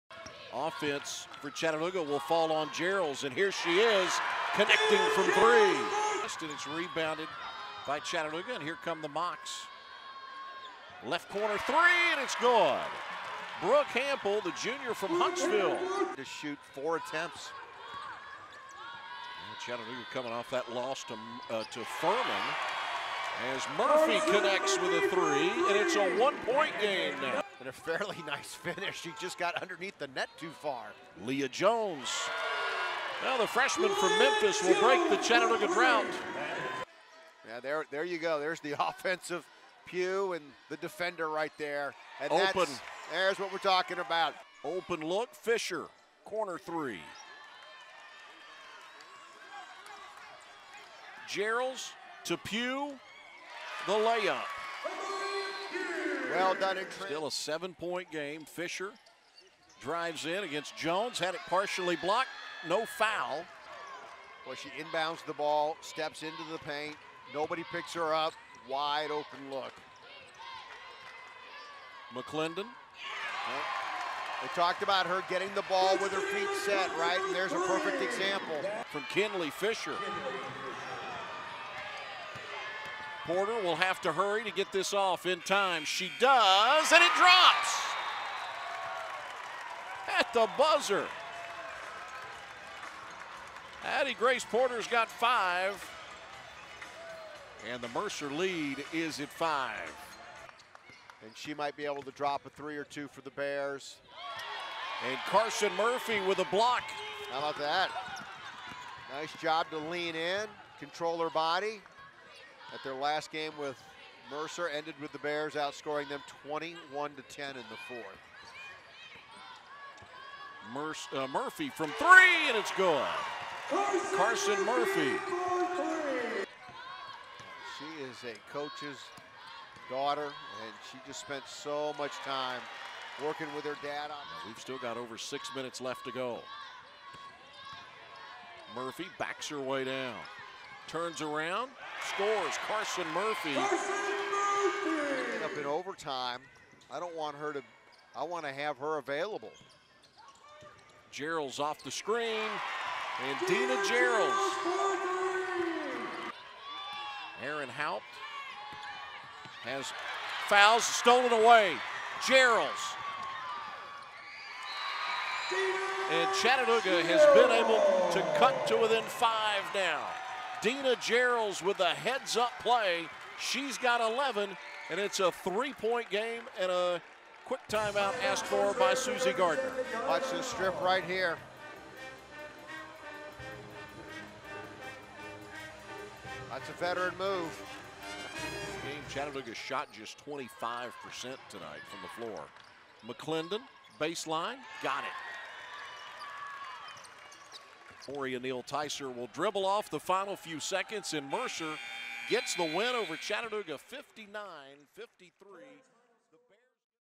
Highlights: Women's Basketball vs. Mercer (2/3/2022)
Chattanooga Women's Basketball faced SoCon opponent Mercer in The McKenzie Arena. Mocs battled to a hard-fought loss 53-59.

Mocs Box Score
Dena Jarrells: 11pts, 2-6 3FG, 5 Reb
Brooke Hampel: 10pts, 2-7 FG, 5 Reb
Karsen Murphy: 9pts, 3-7 FG, 7 Reb
Addie Porter: 8pts, 6-8 FT, 2 Steals